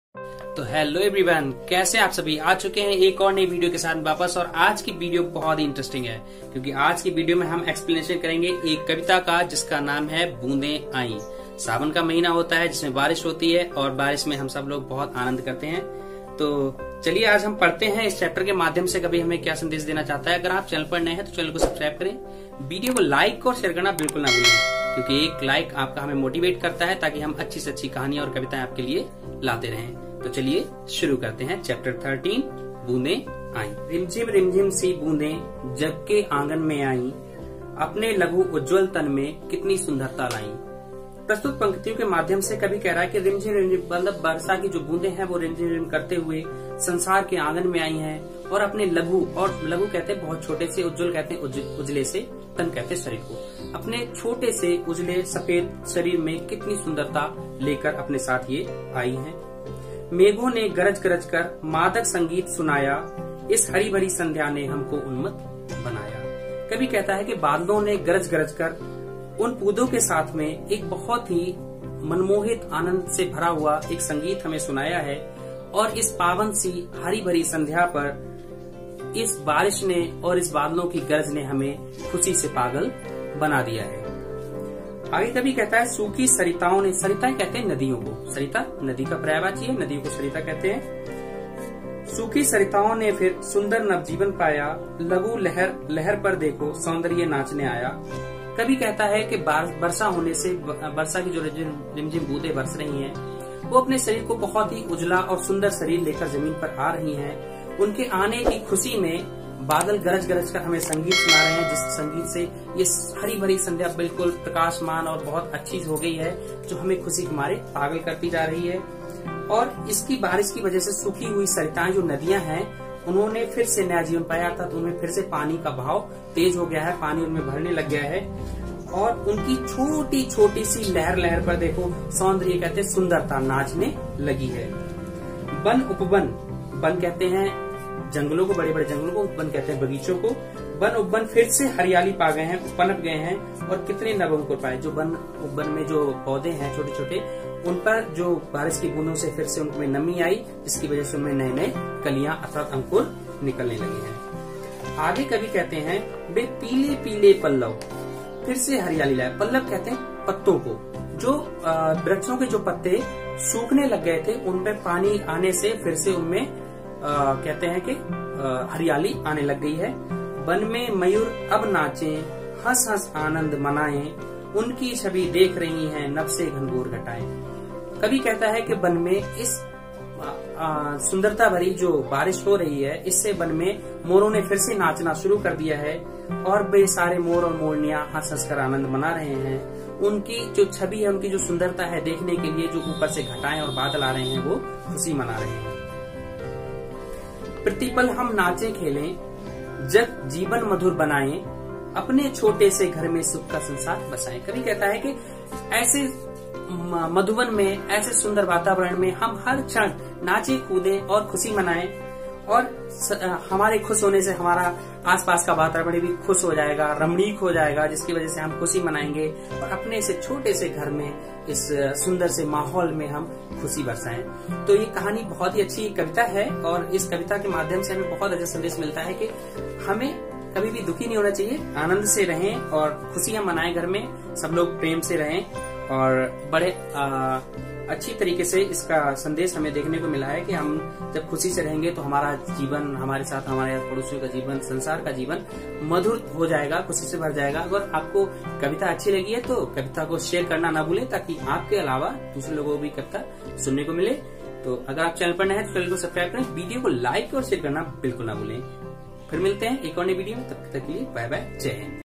बूंदें आईं / Boondein aayin / Rimjhim barsa Pani
Hello everyone kaise hain aap. aaj ki interesting video ki shuruaat karte hain. Rimjhim bunde aain kavita sawan ke mahine ka varnan karti hai poori story k liye dekhiye video.